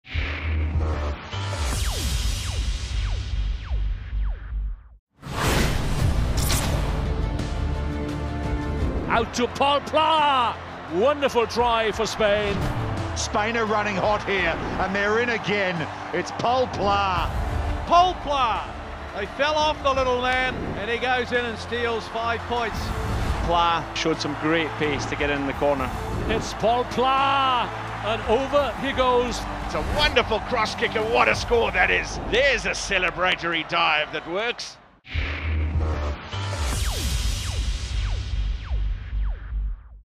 One to watch: pol pla
Spain's Pol Pla has got bags of pace. He's your one to watch at the Singapore sevens this weekend